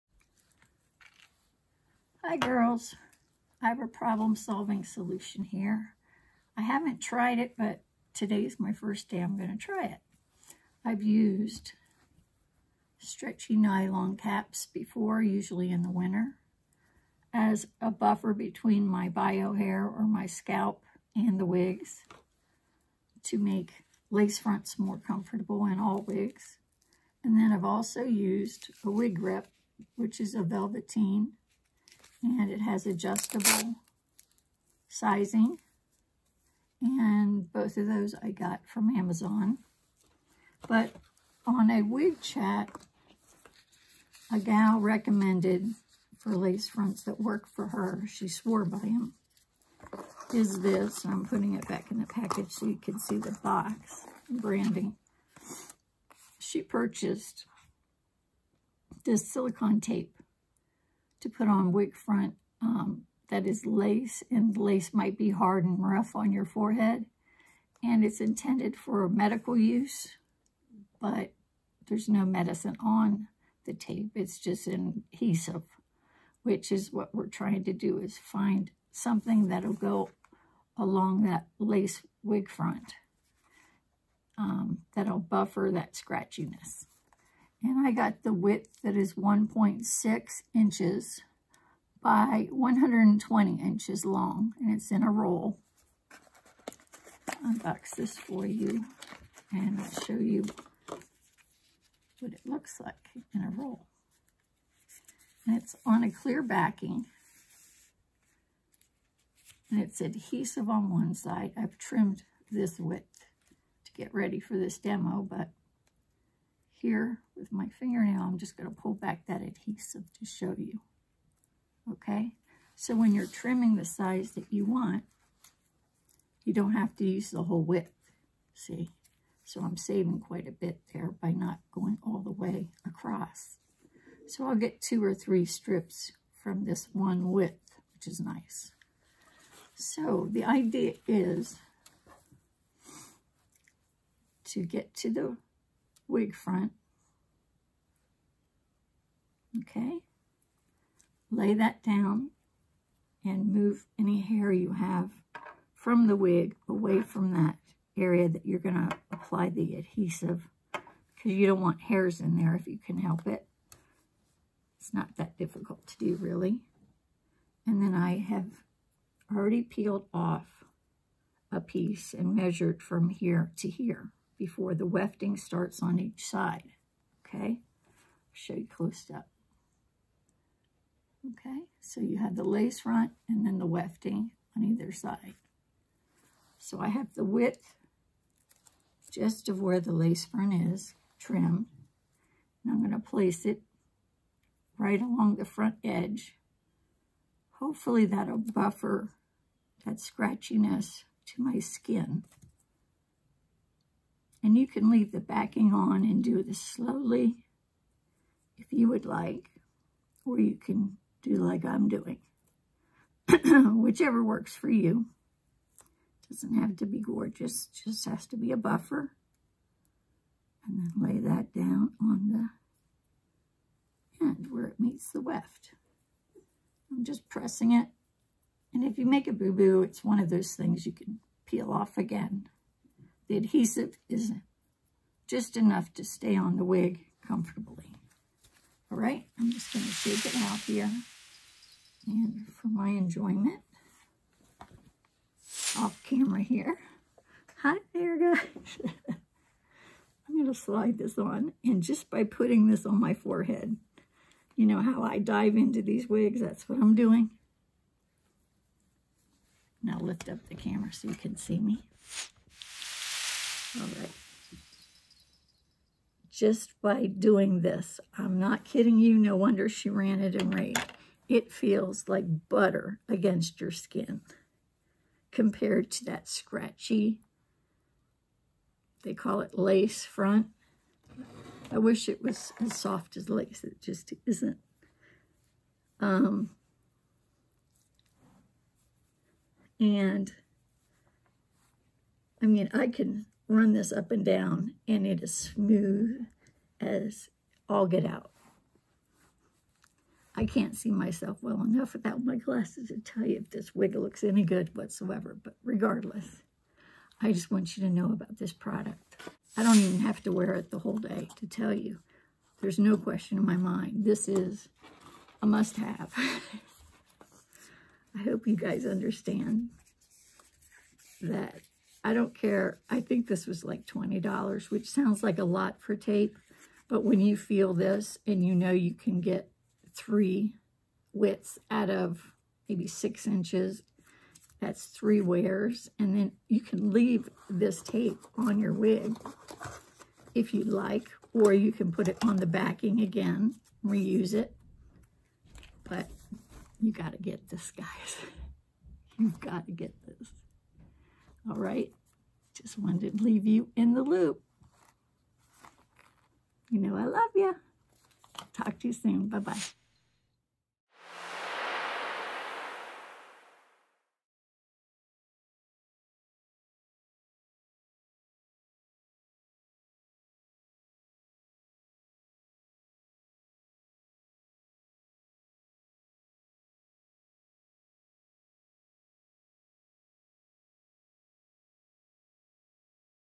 How To Fix A Scratchy Wig - Lace Front
This product is a wig wearer must have! I trialed it and it feels wonderful to soften the rough feel of wig lace front edges on your skin. It’s quick and easy as well and reusable! Watch until the end of the video to get a bonus tip you won’t want to miss. I have Alopecia and wear wigs daily and want to share comfort and inspiration to others with hair loss.